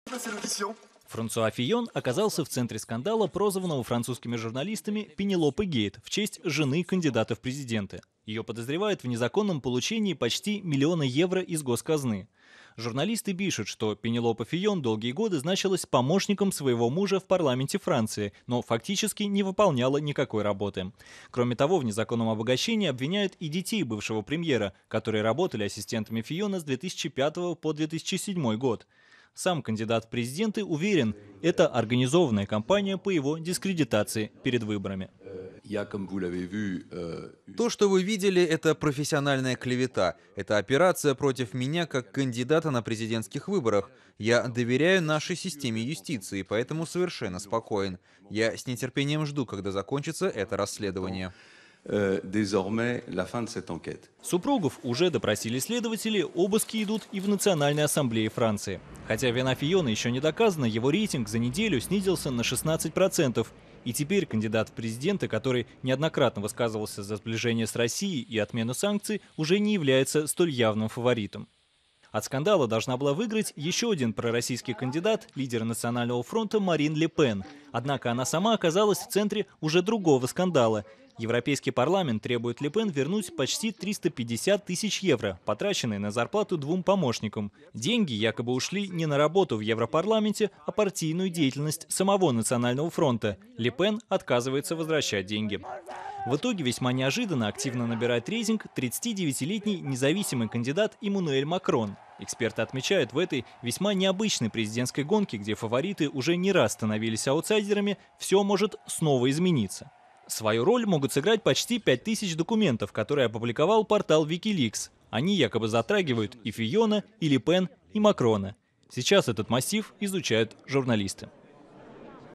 Выборы во Франции: пророссийские кандидаты теряют очки
Одним из лидеров президентской гонки во Франции теперь становится независимый кандидат Эммануэль Макрон.